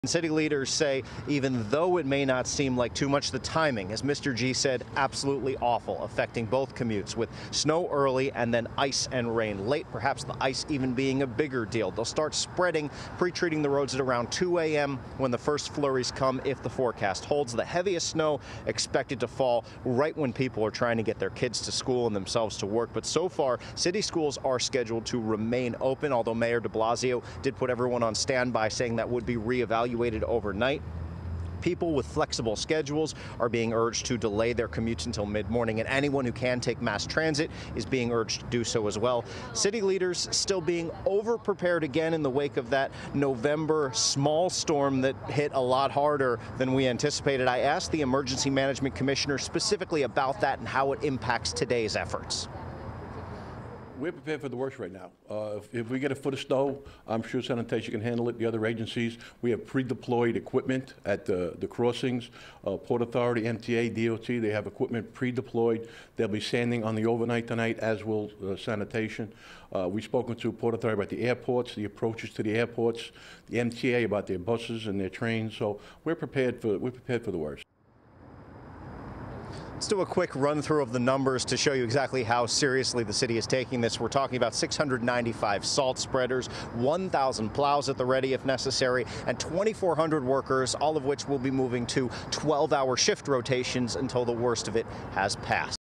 Severe storm to bring snow, sleet, freezing rain, to NY, NJ area
A state of emergency has been declared in New Jersey. Parts of the state could see up to eight inches of snow, Gov. Phil Murphy said.